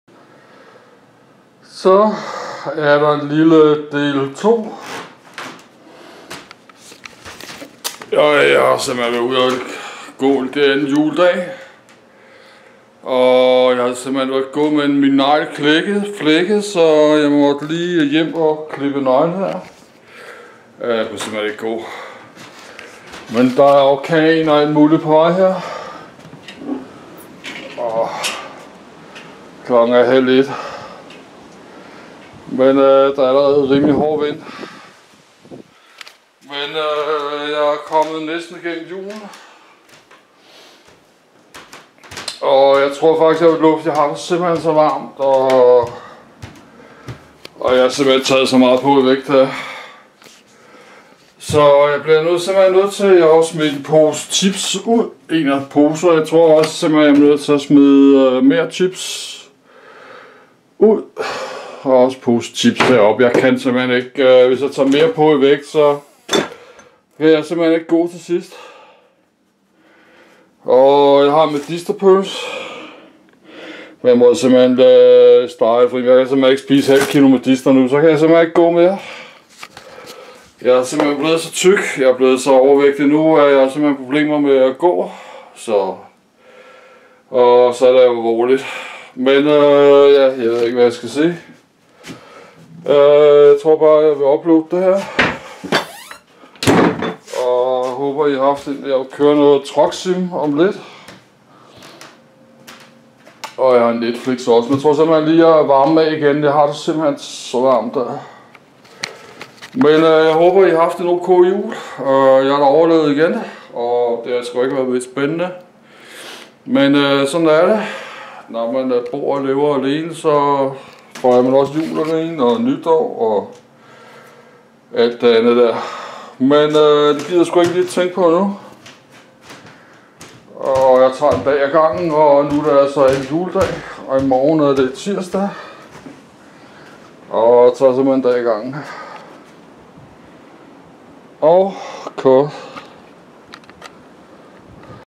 MongoTV_818 - Del 2 - Gåtur 2.Juledag 2016
Del 2 - Jeg kom vist igennem Julen 2016, er 2.Juledag, men er en voldsom STORM eller orkan på vej i aften. Jeg MÅ tabe mig i vægt NU, jeg har svært ved bare at gå tur nu pga. overvægt :-((((